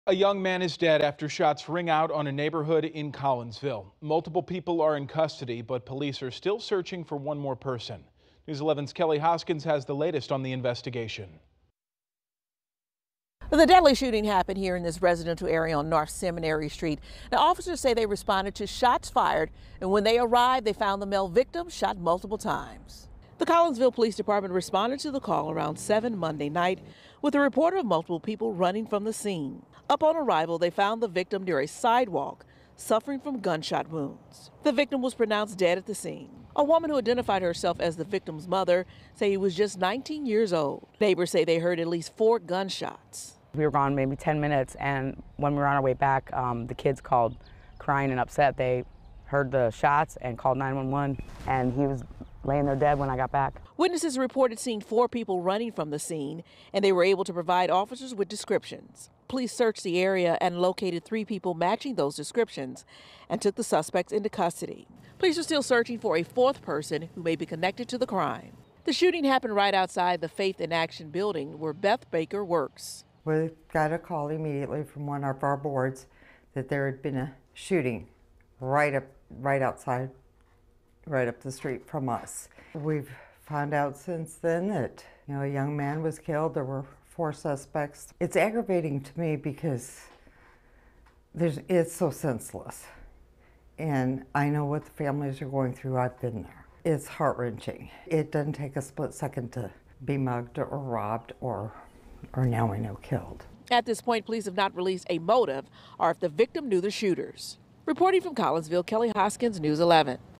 1 man dead in Collinsville shooting; multiple people in custody
A young man is dead after shots rang out Monday night in a Collinsville, Illinois, neighborhood. Multiple people are in custody, but police are still searching for one more suspect.